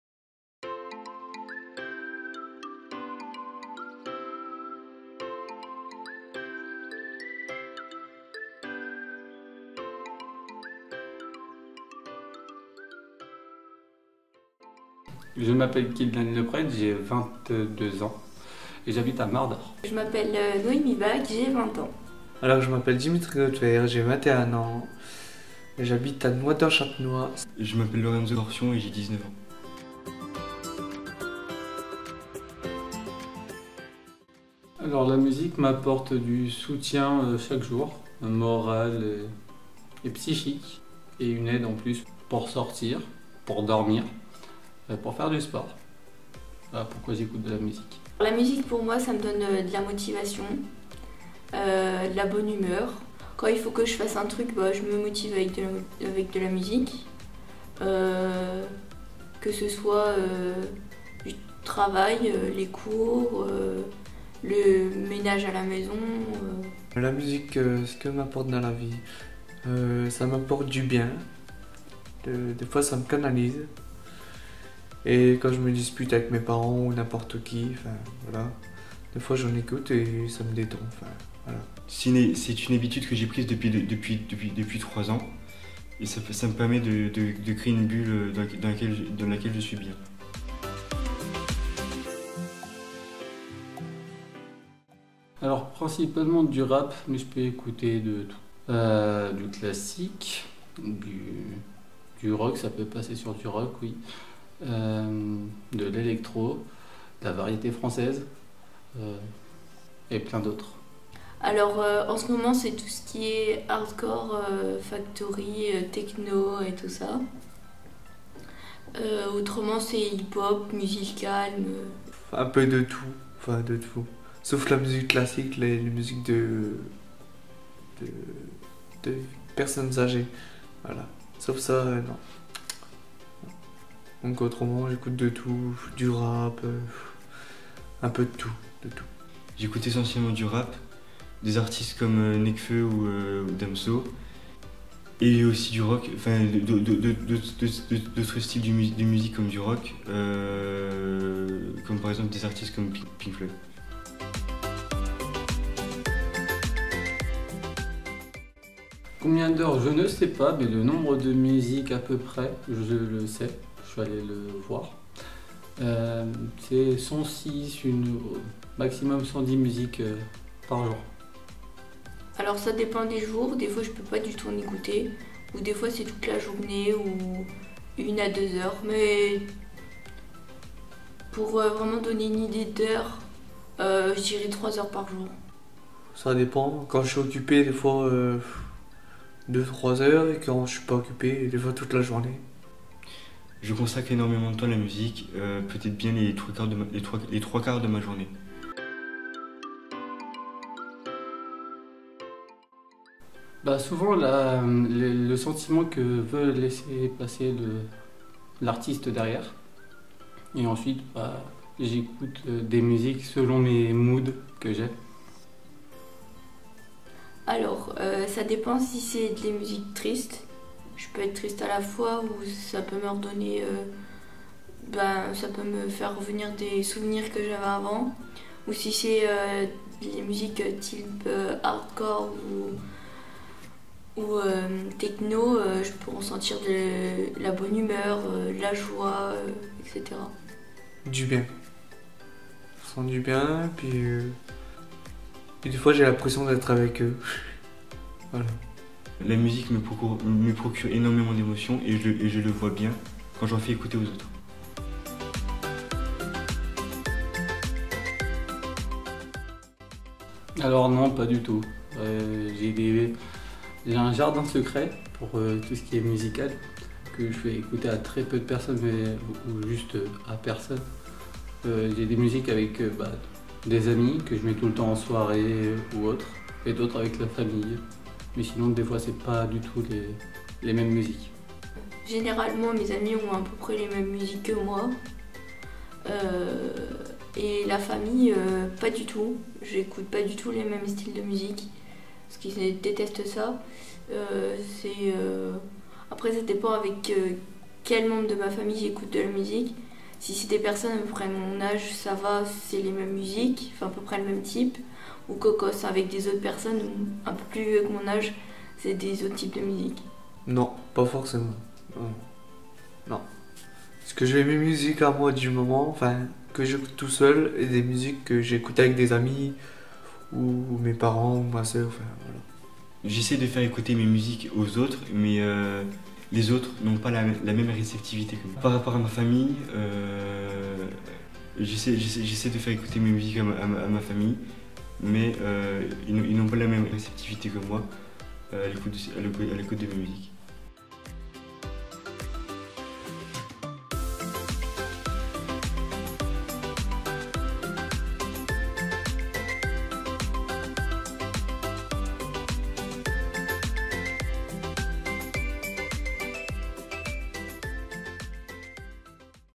Parole de jeunes : L'impact de la musique dans votre vie
⭐️ E-toiles au pays des lumières vous présente la parole de jeunes « L’impact de la musique dans votre vie ». ⭐️

 Le but de ce nouveau projet est de connaître le ressenti que les jeunes ont quand ils écoutent de la musique.

En espérant que ce contenu vous plaira !